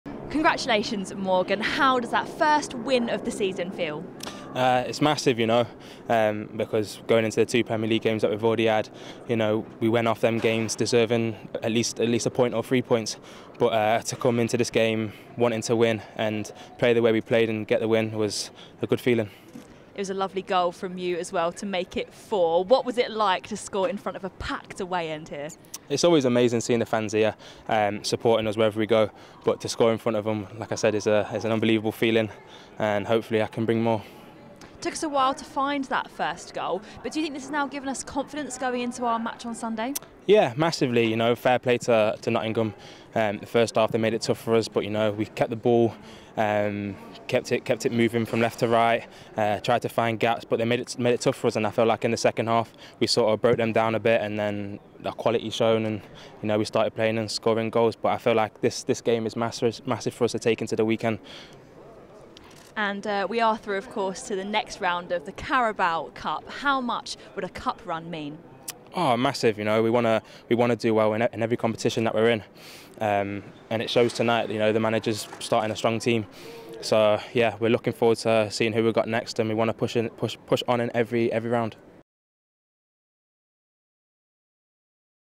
Gibbs-White delighted with his goal and victory over Nottingham Forest
Morgan Gibbs-White spoke with Wolves TV's Gemma Frith following our 4-0 win in the Carabao Cup vs Nottingham Forest at the City Ground.